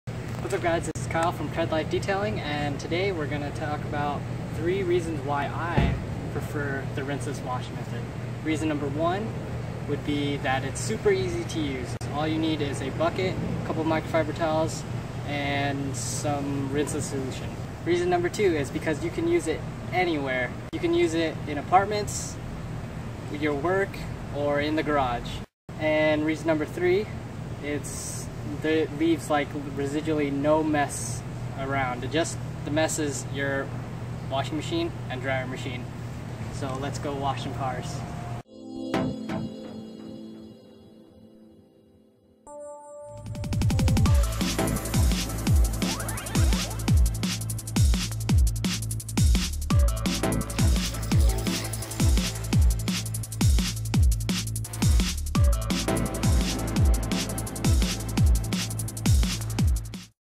TREADLYFE Detailing | 3 Reasons to Love Rinseless Car Wash in 1 Minute
Hey all!

Meet Kyle from TREADLYFE Detailing, he's gonna give his three reasons why he loves the rinseless wash method for getting your car clean with one gallon of water in less than an hour!